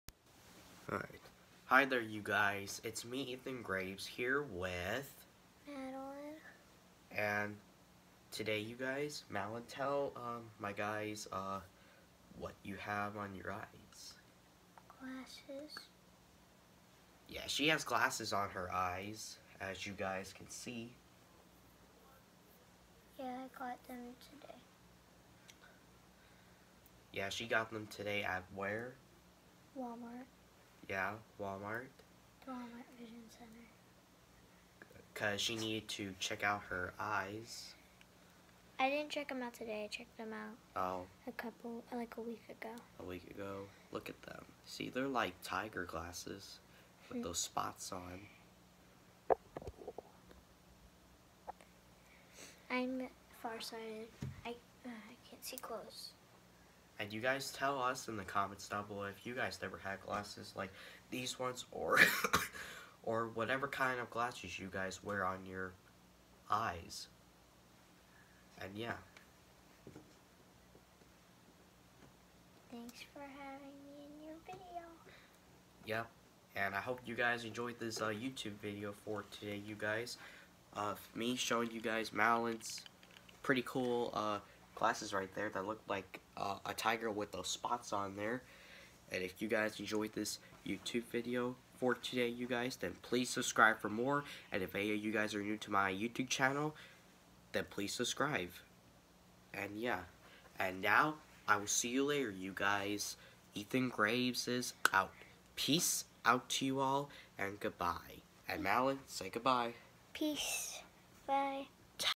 MADELYN NOW HADS GASSES THAT LOOK LIKE A TIGER WITH SOME SPOTS.
MADELYN NOW HADS GASSES THAT LOOK LIKE A TIGER 🐅 WITH SOME SPOTS. Instagram Ethanglewis0204.